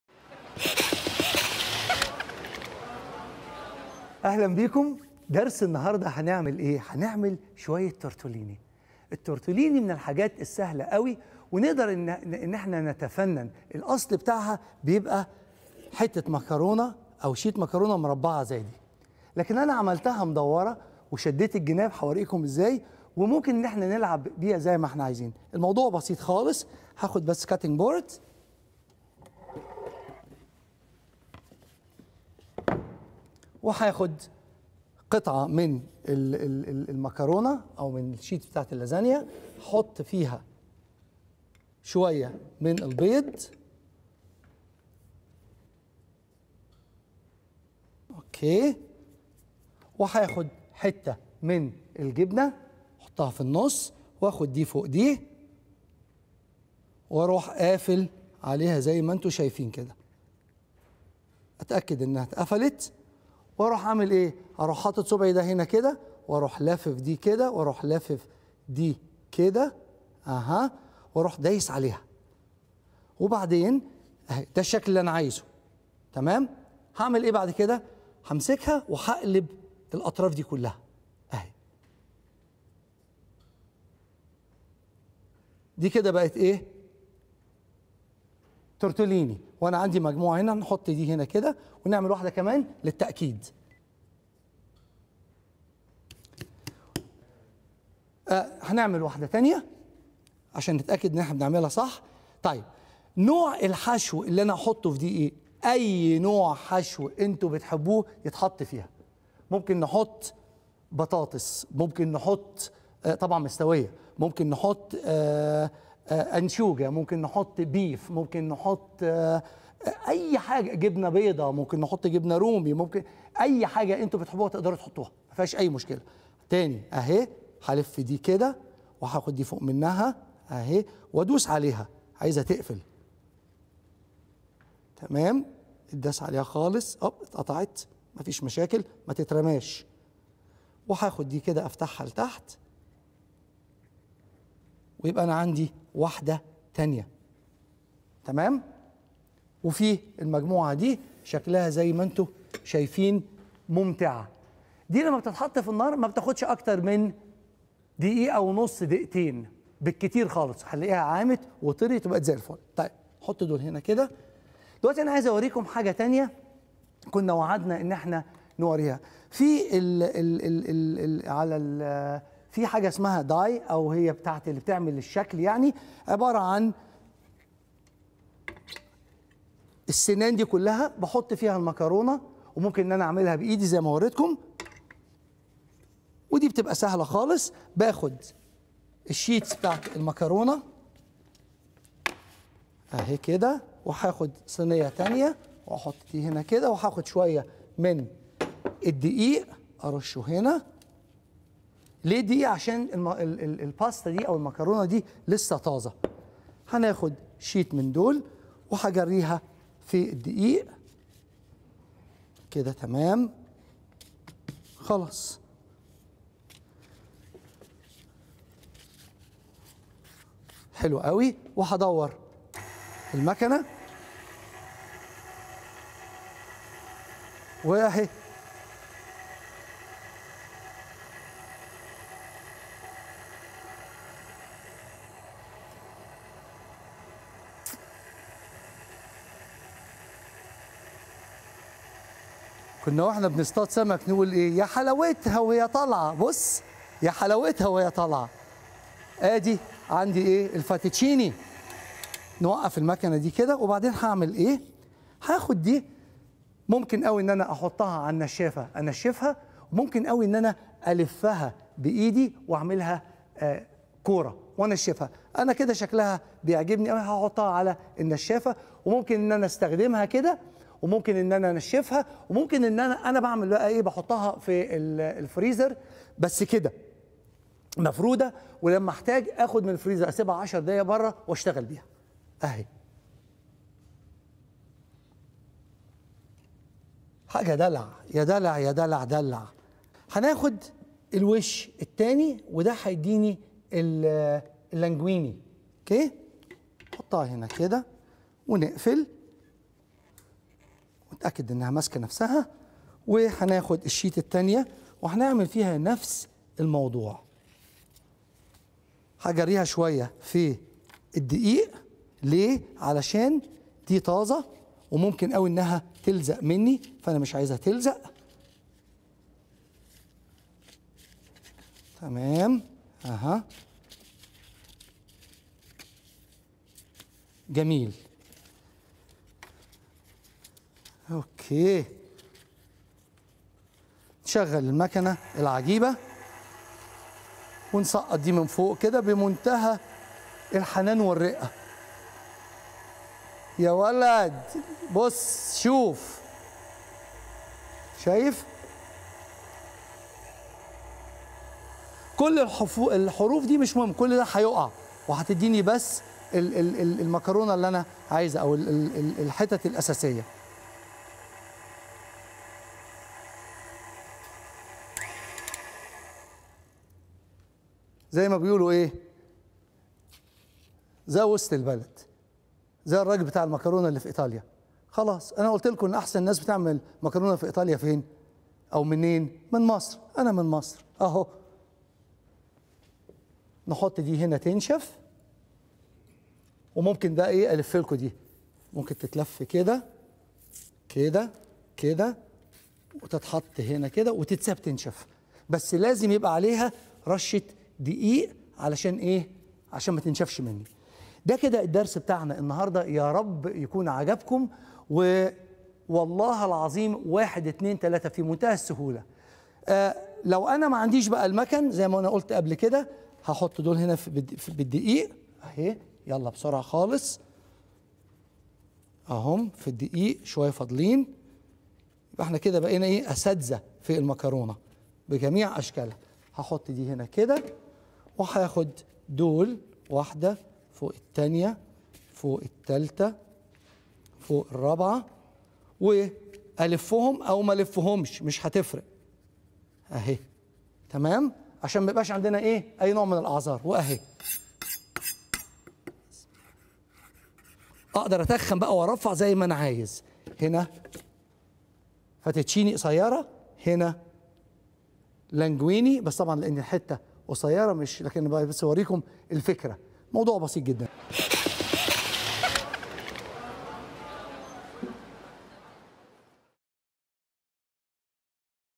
درس: تحضير التورتيليني - 100 مكرونة - فتافيت
كيفية تحضير التورتيليني بـ 6 دقائق